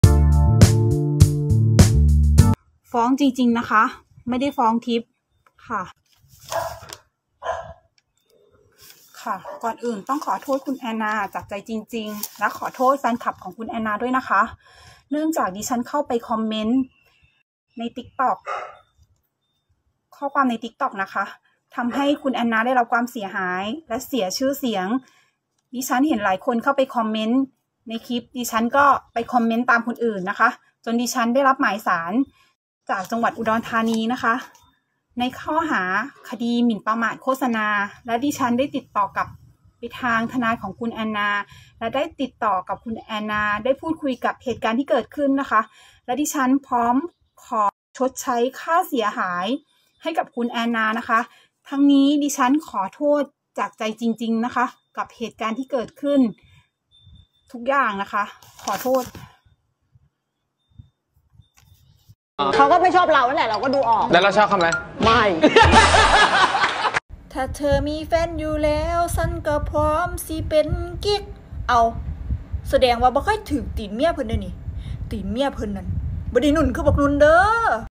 เอาจริง”แอนนา”แฟนเก่า”นุ๊ก ธนดล”ไม่ฟ้องทิพย์ โพสต์เกรียนคีย์บอร์ดขอโทษชดใช้ค่าเสียหาย#แอนนา#นุ๊กปาย
เอาจริง”แอนนา”แฟนเก่า”นุ๊ก ธนดล”ไม่ฟ้องทิพย์ โพสต์เกรียนคีย์บอร์ดขอโทษชดใช้ค่าเสียหาย